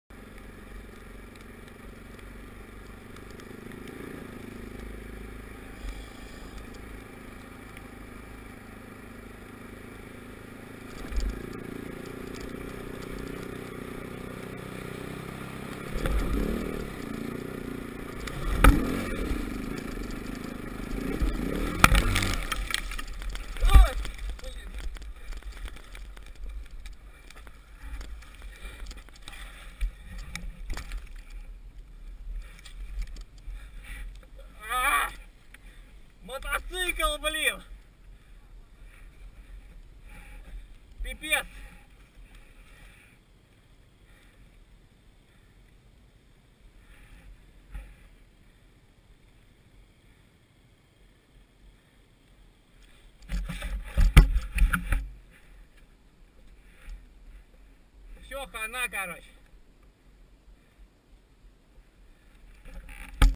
Russian Rider's Bike Breaks the Ice || ViralHog
Occurred on January 4, 2019 / Moscow, Russia

"I was driving my bike on a frozen Moscow channel and fell through the ice. The bike needed to be serviced, but both it and I am okay."